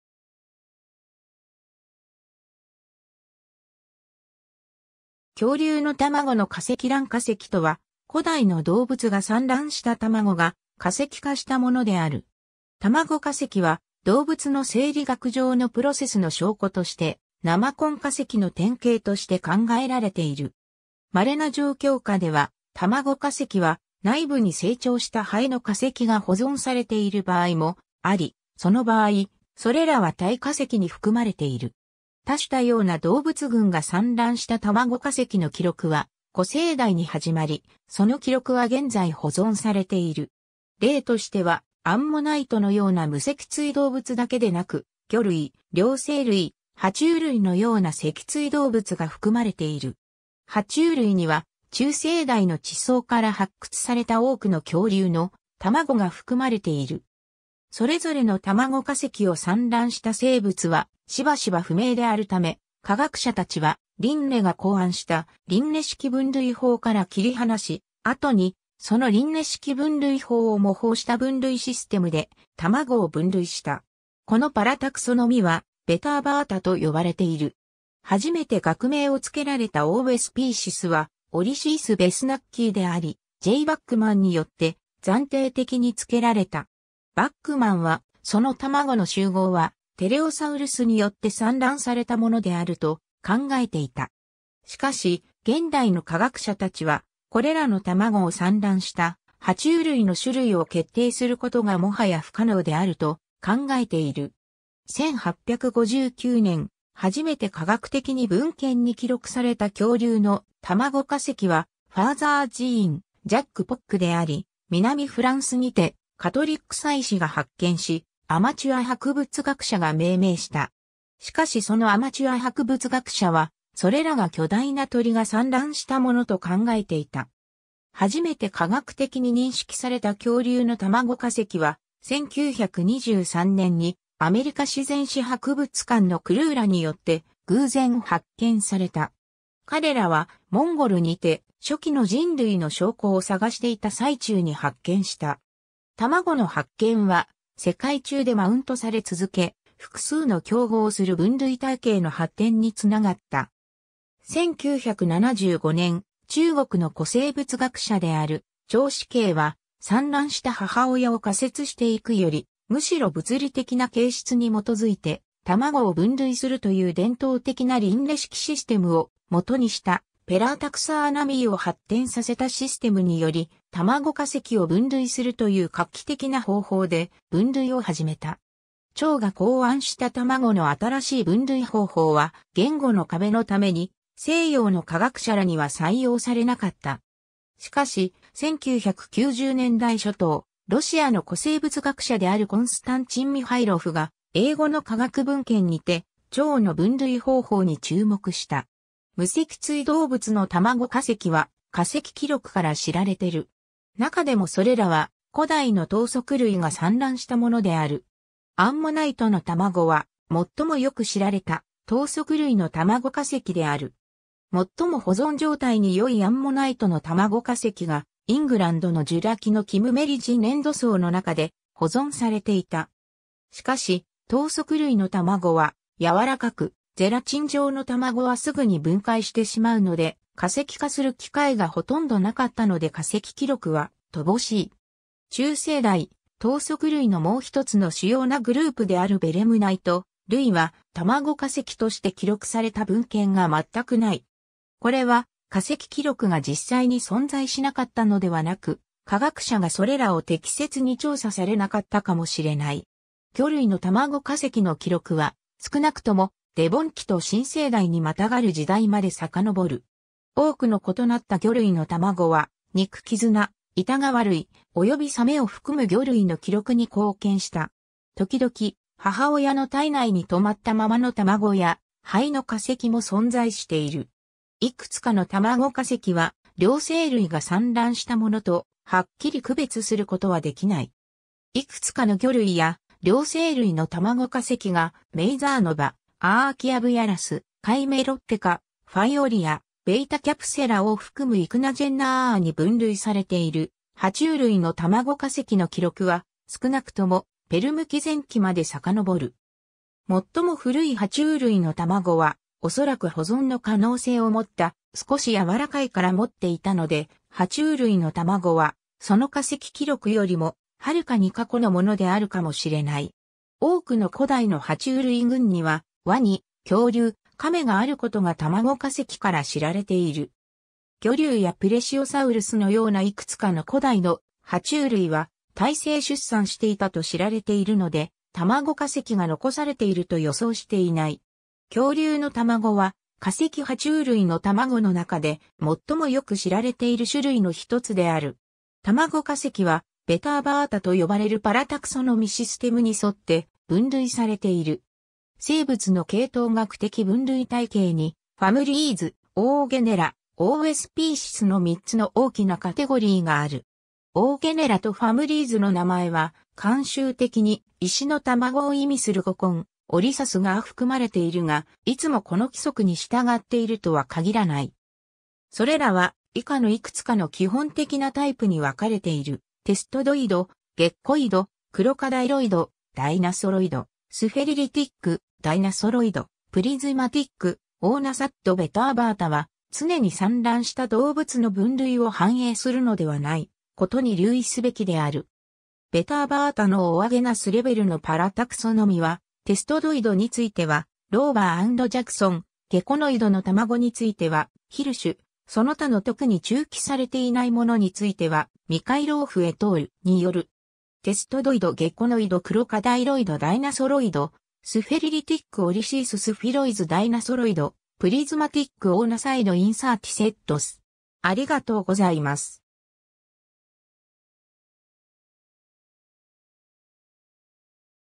卵化石
恐竜の卵の化石（インドローダ恐竜と化石公園にて展示） 卵化石（らんかせき）とは、古代の動物が産卵した卵が化石化したものである。
卵化石は動物の生理学上のプロセスの証拠として、生痕化石の典型として考えられている。
まれな状況下では、卵化石は、内部に成長した胚の化石が保存されている場合もあり、その場合、それらは体化石に含まれている。
多種多様な動物群が産卵した卵化石の記録は古生代に始まり、その記録は現在保存されている。
例としては、アンモナイトのような無脊椎動物だけでなく、魚類、両生類、爬虫類のような脊椎動物が含まれている。
爬虫類には、中生代の地層から発掘された多くの恐竜の卵が含まれている。
それぞれの卵化石を産卵した生物は、しばしば不明であるため、科学者たちは、リンネが考案したリンネ式分類法から切り離し、後にそのリンネ式分類法を模倣した分類システムで卵を分類した。
このパラタクソノミ（parataxonomy）はveterovataと呼ばれている。
 初めて学名をつけられた oospecies は、Oolithes bathonicaeであり、J.バックマンによって暫定的につけられた。
バックマンは、その卵の集合は、テレオサウルスによって産卵されたものであると考えていた。
しかし、現代の科学者たちは、これらの卵を産卵した爬虫類の種類を決定することがもはや不可能であると考えている。
 1859年、初めて科学的に文献に記録された恐竜の卵化石は、Father Jean-Jacques Poechであり、南フランスにてカトリック祭司が発見し、アマチュア博物学者が命名した。
しかしそのアマチュア博物学者は、それらが巨大な鳥が産卵した物と考えていた。
 初めて科学的に認識された恐竜の卵化石は、1923年にアメリカ自然史博物館のクルーらによって偶然発見された。
彼らは、モンゴルにて初期の人類の証拠を探していた最中に発見した。
卵の発見は、世界中でマウントされ続け、複数の競合する分類体系の発展につながった。
1975年、中国の古生物学者である趙資奎は、産卵した母親を仮説していくよりむしろ物理的な形質に基づいて卵を分類するという伝統的なリンネ式システムを基にしたparataxonomyを発展させたシステムにより卵化石を分類するという画期的な方法で分類を始めた。
趙が考案した卵の新しい分類方法は、言語の壁のために西洋の科学者らには採用されなかった。
しかし、1990年代初頭、ロシアの古生物学者であるコンスタンチンミハイロフが英語の科学文献にて趙の分類方法に注目した。
 無脊椎動物の卵化石は、化石記録から知られてる。
なかでもそれらは古代の頭足類が産卵したものである。
アンモナイトの卵は、最もよく知られた頭足類の卵化石である。
最も保存状態によいアンモナイトの卵化石が、イングランドのジュラ紀のキムメリッジ粘土層の中で保存されていた。
しかし、頭足類の卵は、柔らかくゼラチン状の卵はすぐに分解してしまうので、化石化する機会がほとんどなかったので化石記録は乏しい。
中生代、頭足類のもう一つの主要なグループであるベレムナイト類は、卵化石として記録された文献がまったくない。
これは、化石記録が実際に存在しなかったのではなく、科学者がそれらを適切に調査されなかったかもしれない。
 魚類の卵化石の記録は、少なくともデボン紀と新生代にまたがる時代までさかのぼる。
多くの異なった魚類の卵は、肉鰭綱、板皮類、およびサメを含む魚類の記録に貢献した。
ときどき、母親の体内に止まったままの卵や胚の化石も存在している。
いくつかの卵化石は、両生類が産卵したものとはっきり区別することはできない。
 いくつかの魚類や両生類の卵化石がMazonova、 Archaeoovulus、Chimaerotheca、Fayolia、Vetacapsulaを含むichnogeneraに分類されている 爬虫類の卵化石の記録は、少なくともペルム紀前期まで遡る。
最も古い爬虫類の卵は、おそらく保存の可能性を持った少し柔らかい殻持っていたので、爬虫類の卵は、その化石記録よりもはるかに過去のものであるかもしれない。
多くの古代の爬虫類群には、ワニ、恐竜、カメがあることが卵化石から知られている。
魚竜やプレシオサウルス のようないくつかの古代の爬虫類は、胎生出産していたと知られているので、卵化石が残されていると予想していない。
恐竜の卵は化石爬虫類の卵の中で最もよく知られている種類の一つである。
 卵化石はveterovataと呼ばれるパ...